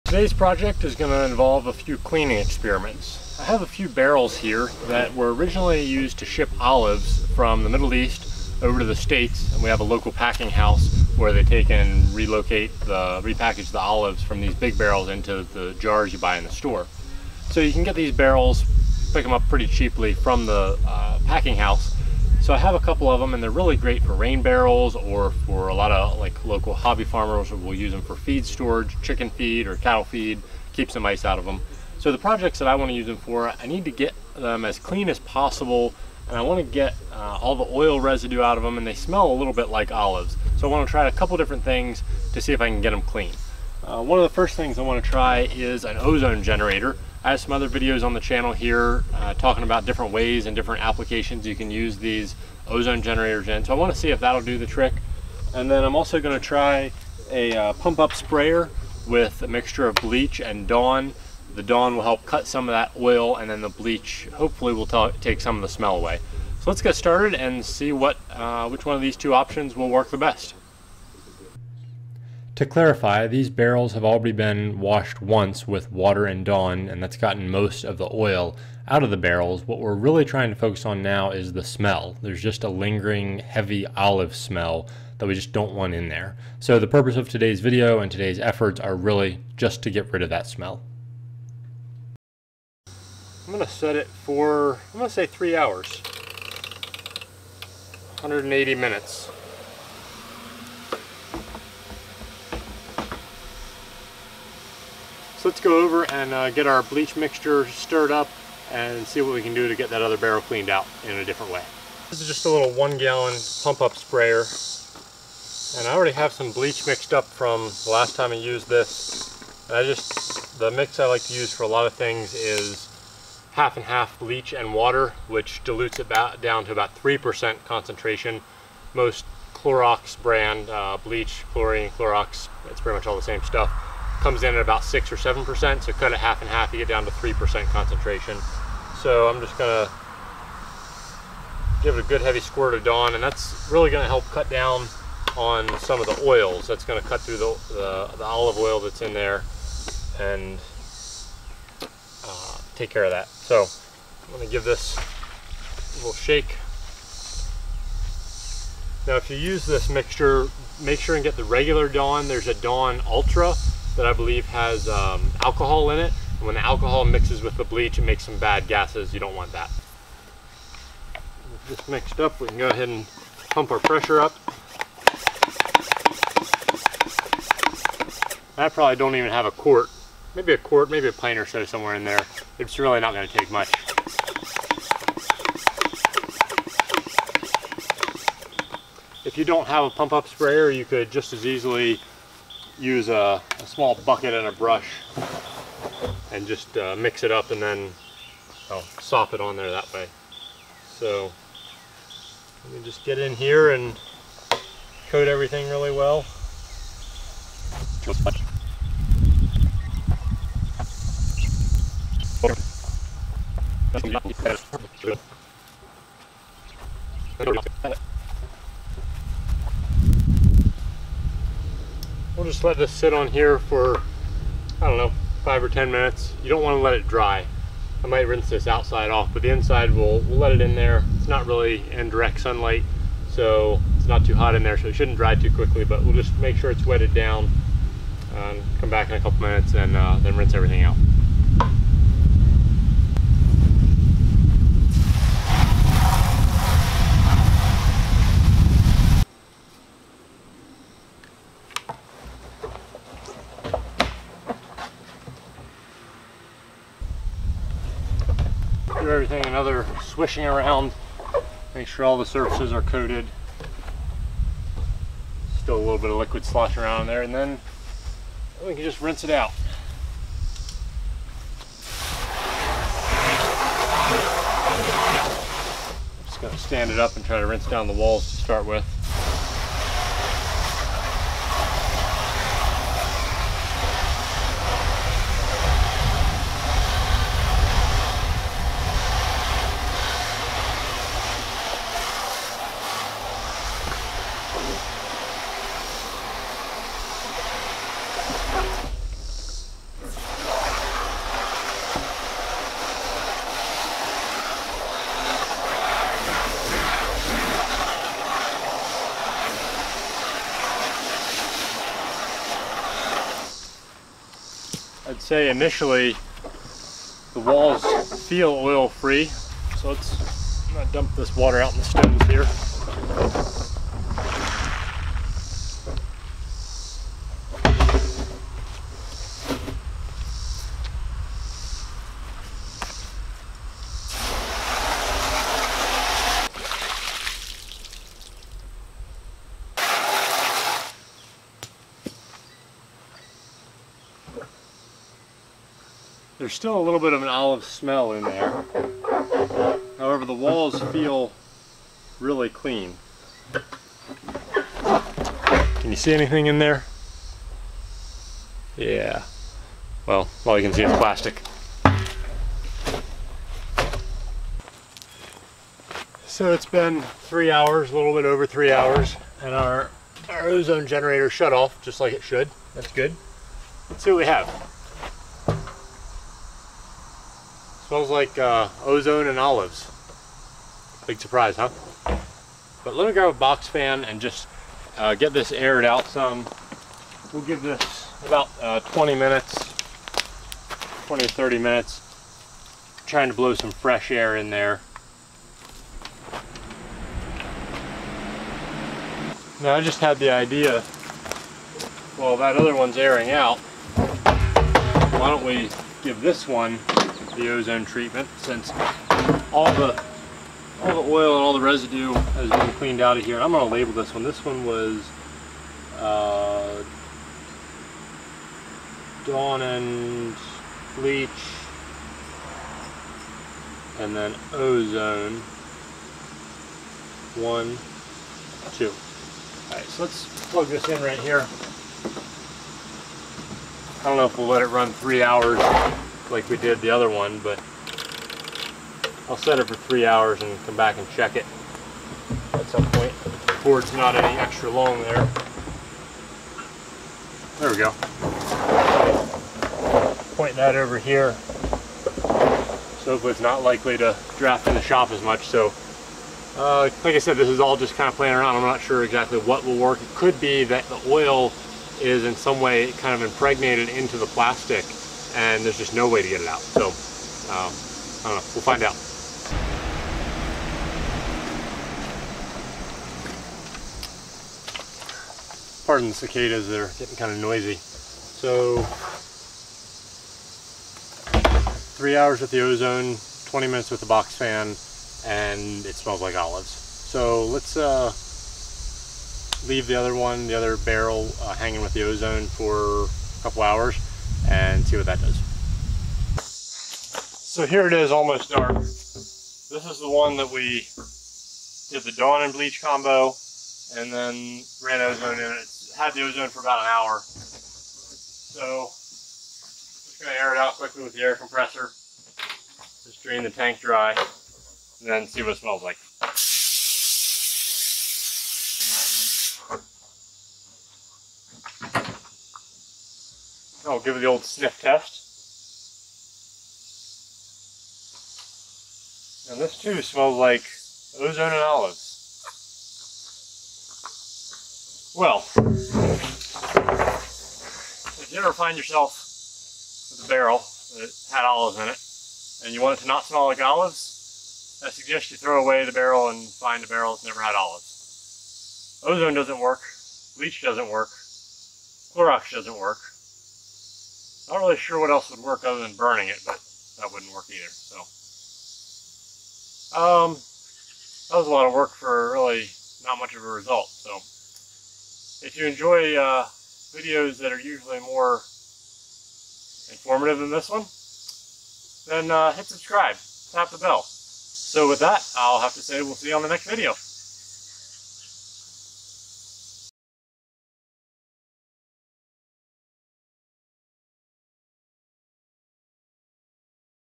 Will Ozone Kill Olive Stink?
Ozone is designed to remove some heavy odors, but will it work against olives?